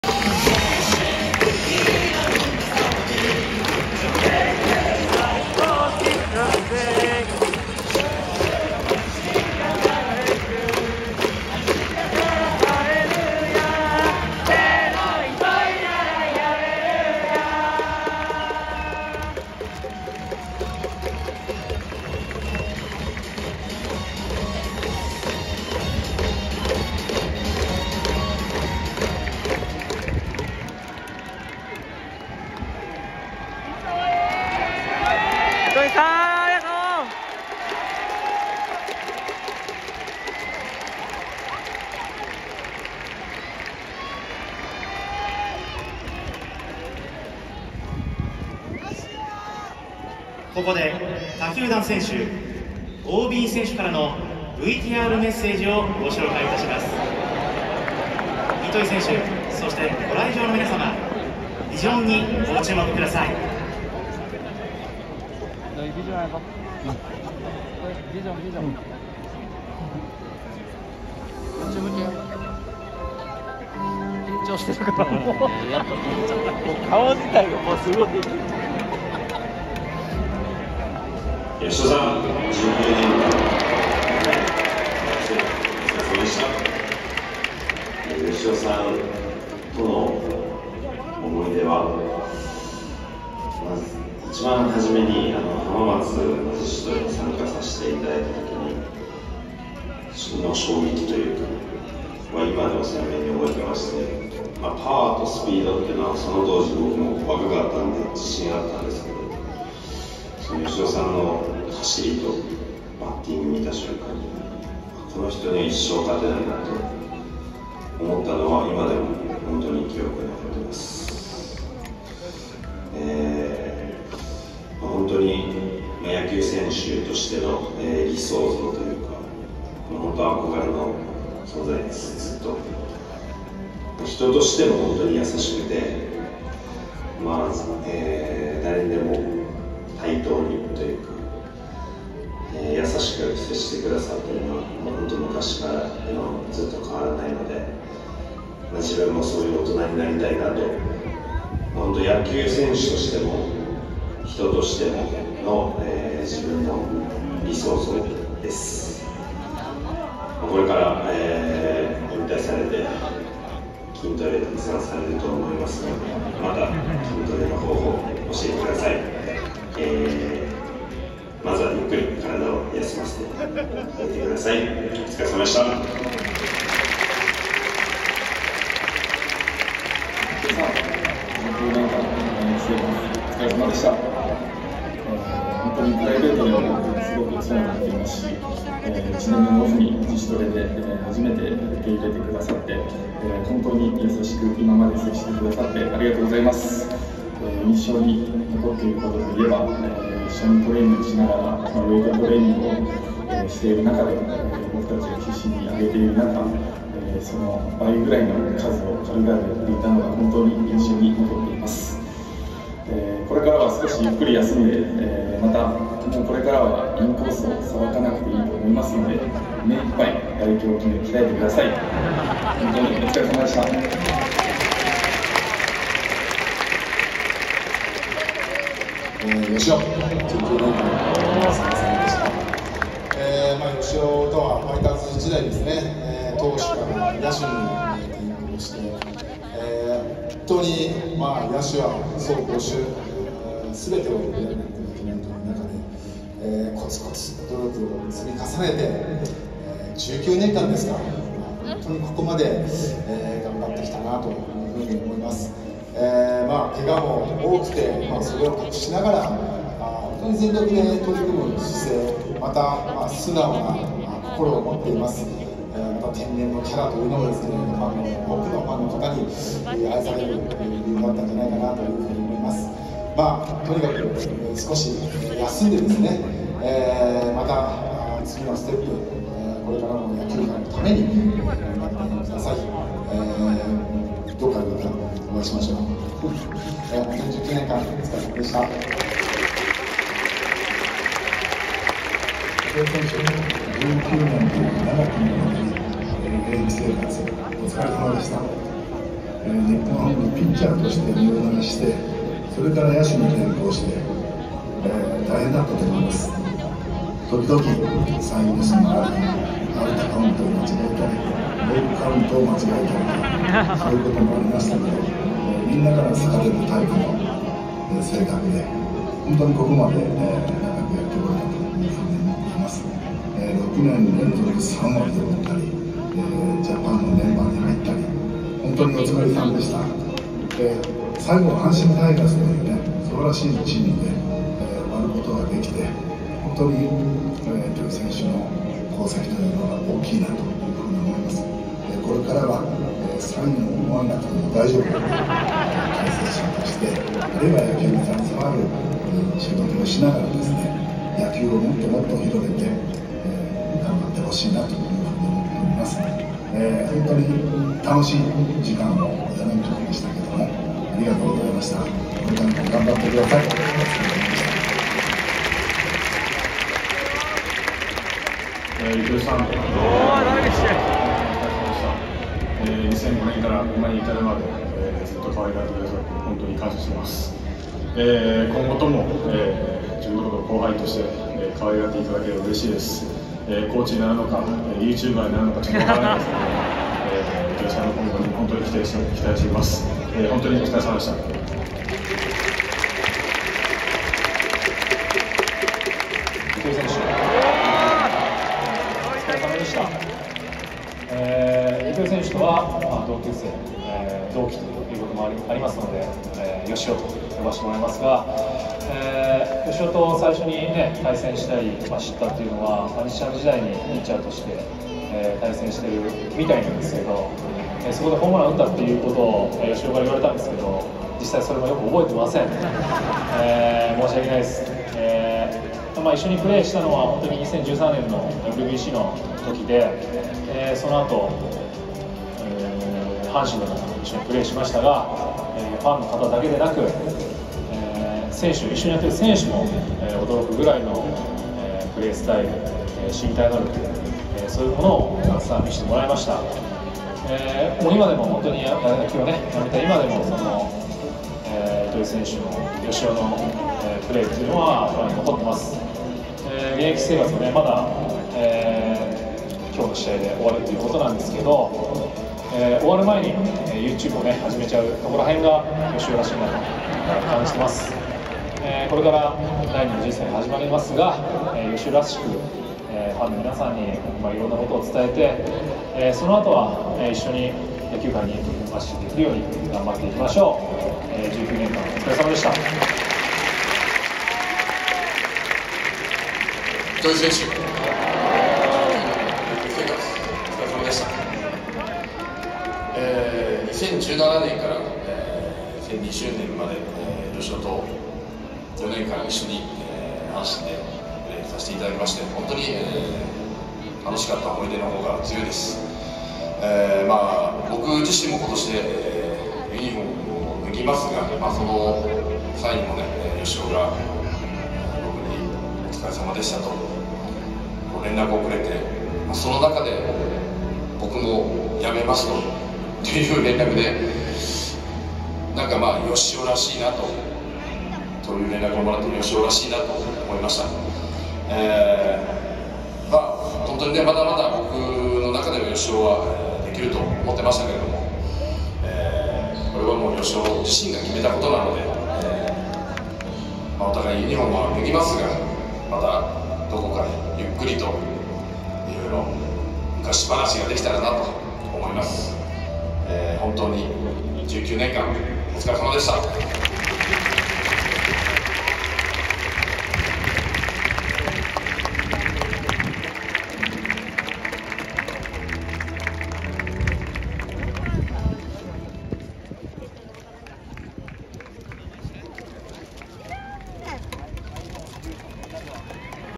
【ノーカット】糸井嘉男へ大物プロ野球選手7人の贈るメッセージ！超人伝説すぎるエピソードが溢れ出る！ 〜糸井嘉男引退セレモニー②〜
DMでよろしくお願いします！